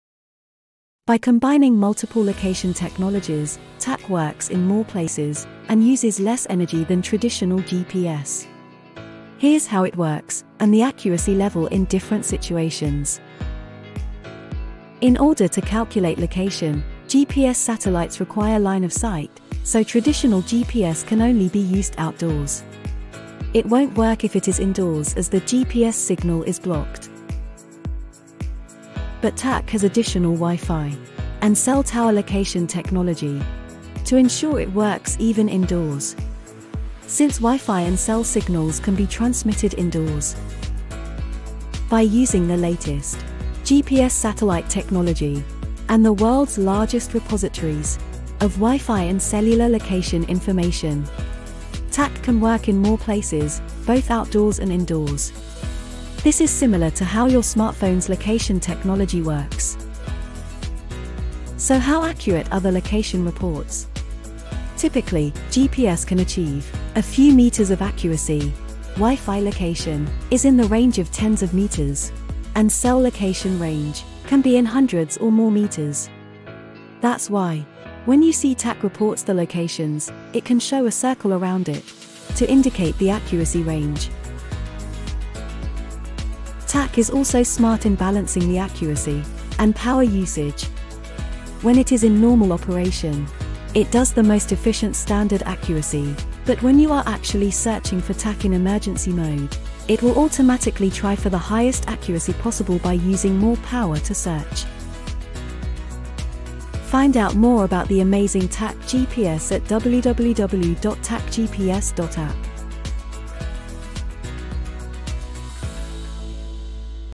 Tack GPS location tech and accuracy explained
Tack combines several location technologies, so it can work in more places and consume less energy than traditional GPS methods. Here's how it works and the accuracy levels.

In order to calculate location, GPS satellites require line of sight, so traditional GPS can only be used outdoors. It won't work if it is indoors as the GPS signal is blocked.

But Tack has additional WiFi and cell tower location technology to ensure it works even indoors,
since WIFI and cell signals can be transmitted indoors.
By using the latest GPS satellite technology and the world's largest repositories of wifi and cellular location information, Tack can work in more places both outdoors and indoors, This is similar to how your smartphone's location technology works.

So how accurate are the location reports, typically, GPS can achieve a few meters of accuracy, WiFi location is in the range of tens of meters, and cell location range can be in hundreds or more meters. That's why when you see Tack reports the locations, it can show a circle around it to indicate the accuracy range.

Tack is also intelligent in balancing the accuracy and power usage, when it is in normal operation, it uses the most efficient normal accuracy mode, but when you are actually searching for Tack using emergency mode, it will automatically try for the highest accuracy possible by using more power to search.